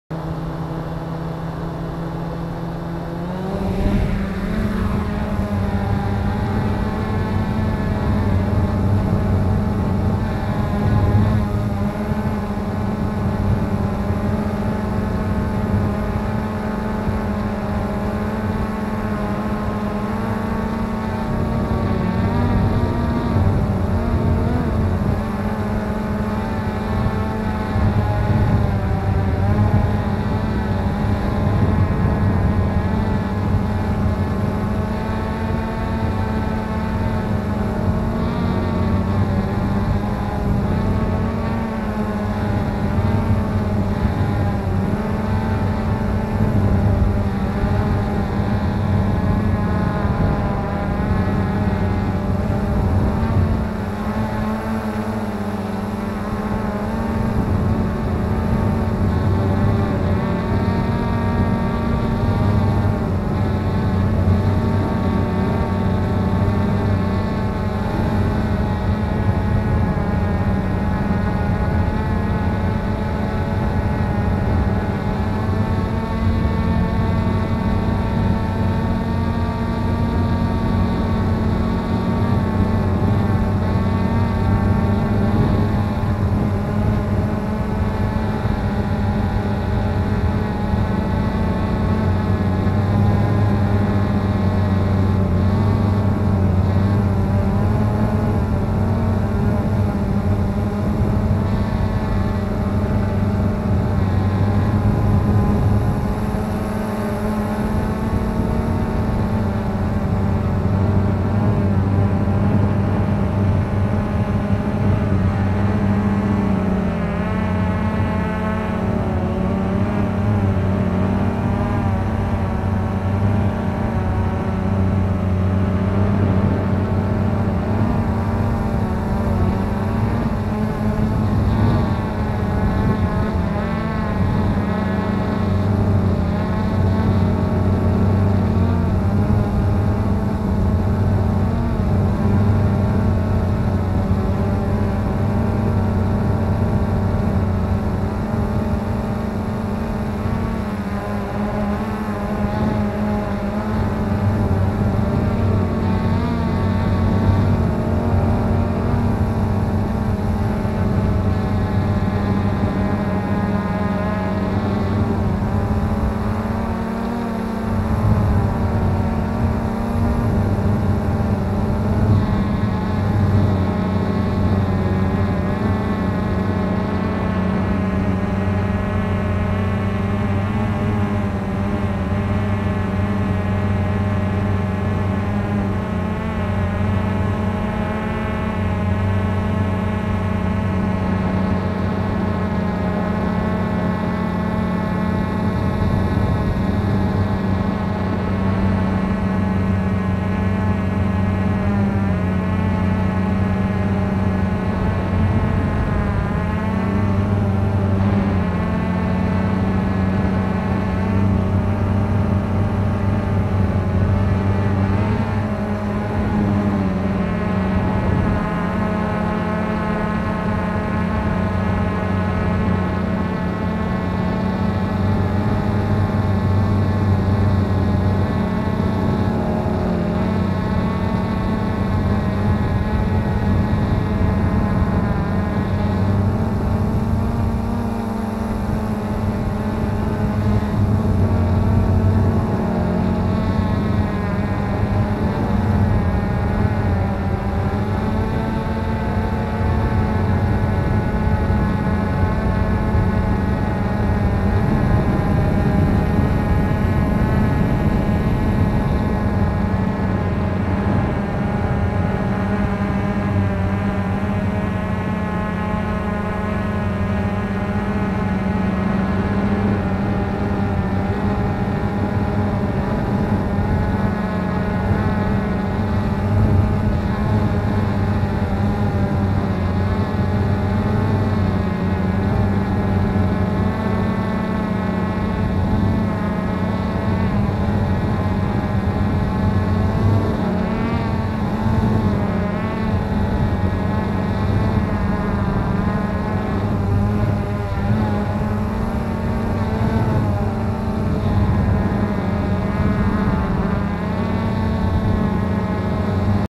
Teste Cheerson CX-20 - Walkera G-2D #1
Primeiros testes com o Gimbal

Santo Ângelo - RS
14/10/16

Drone Cheerson CX-20
Gimbal Walkera G-2D
Câmera SJ4000